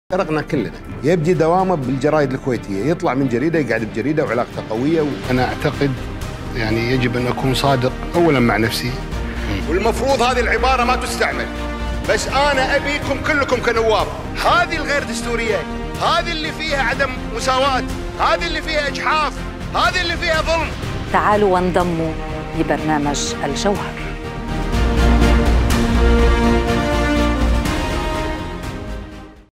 اكتشف الجوهر
‏‎تدرب على مهارات الحوار مع أبرز الإعلاميين وتعلم كيف: تبحث وتسأل وتحاور شخصيات ريادية ساهمت بتكوين جوهر نهضة الكويت والدول العربية.
‏‎📌الورشة الأولى: 9 - 13 يونيو
تدريب وإشراف الإعلامية جيزال خوري.
📌حلقة الحوار: 15 - 16 يونيو
مع ضيف الحلقة: النائب السابق ورئيس مجلس إدارة غرفة تجارة وصناعة الكويت السيد محمد جاسم الصقر.
📌الفئة العمرية: 16 - 40.